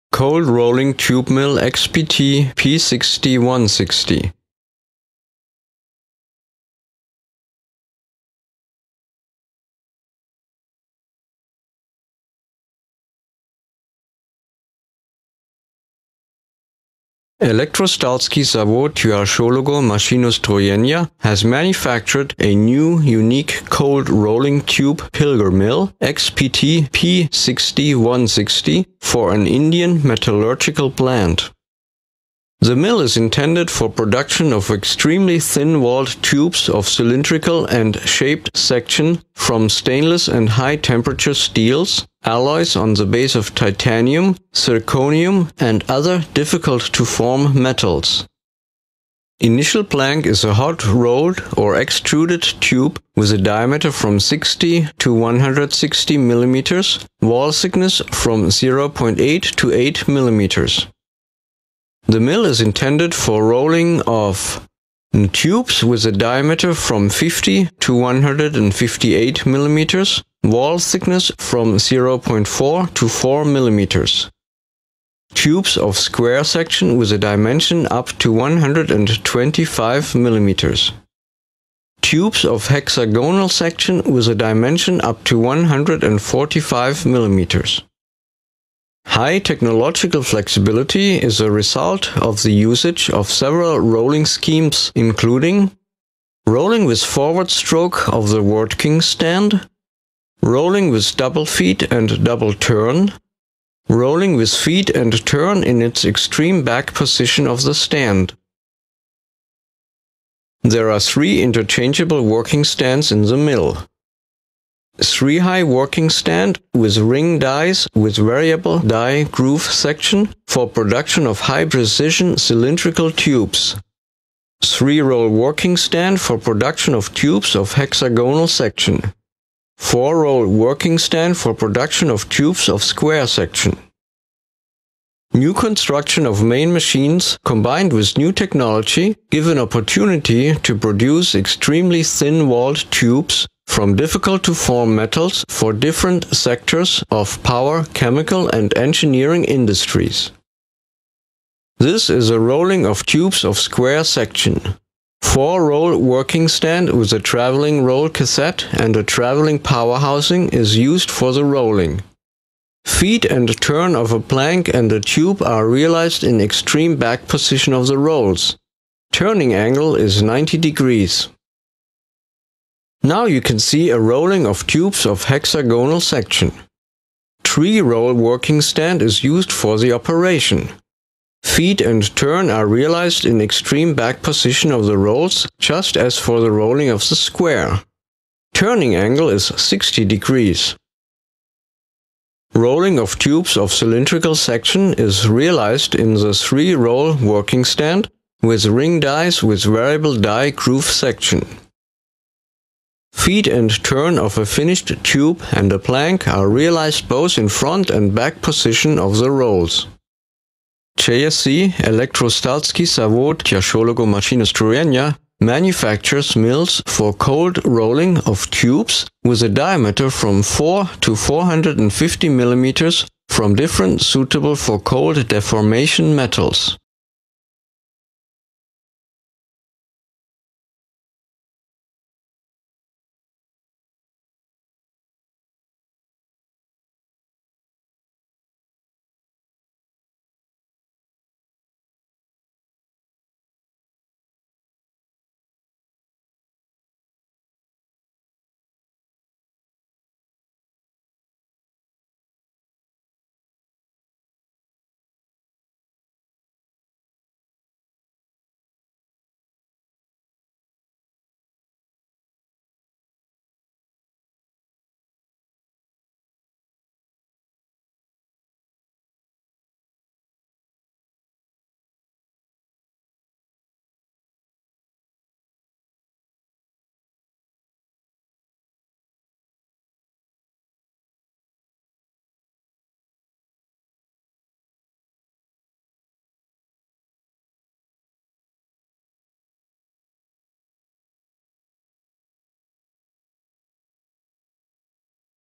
Cold rolling tube mill P 60-160 (CRTM 60-160)
Cold rolling tube mill P 60-160

New universal cold pilger mill 60-160 is intended for production of extremely thin walled tubes of cylindrical, hexagonal or square section from high strength hardly deformed materials such as stainless and heatproof steels, titanium, zirconium and their alloys.Кассета (4-х роликовая, квадрат)
High technological mobility is achieved by the use of the following technological rolling schemes:
rolling with double feed and double turn;
rolling at forward stroke of the working stand;
 rolling with feed and turn in the extreme back position of the working stand only.
To extend technological possibilities of the mill it is possible to equip it with the following interchangeable working stands:
3-high stand with ring passes having variable groove section is intended for production of high precision cylindrical tubes;
3-high stand is intended for production of hexagonal tubes
 4-high stand is intended for production of square tubes.
The mill consists of the following equipment:

1. Equipment of a working stand with a driveТрехроликовая клеть (шестигранник)
Equipment of a working stand with a drive is a complex of mechanisms performing basic operations of the tube production process:
three-roller working stand;
four-roller working stand;
working stand displacement drive;
 housing;
 lever system;
intermediate vice;
 inlet vice;
protection devices;
devices for rolling lubricant feed and collection.
2.      Feed section equipment
Feed section equipment is a complex of mechanisms acting on a billet and a bar with a mandrel intended for converting of continuous rotation of feed and turn actuators into the impulse one:
 inlet vice with guides;Трехроликовая клеть (сфера)
mandrel bar fixing mechanism;
 lubrication feeding device.
3.      Loading table
Loading table is a complex of mechanisms providing receiving, layout of billets and their loading:
sloping skid;
feed control device;
receiving roll table.
4.      Finished tubes receiving table
dead roller table ;ХПТ 60-160 готовые трубы
ejector;
sloping skid.
5.      Hydrosystem
a reservoir with two pumping units;
separator gas accumulator;
control valve panels.
6.      Lubrication system
7.      Electrical facilities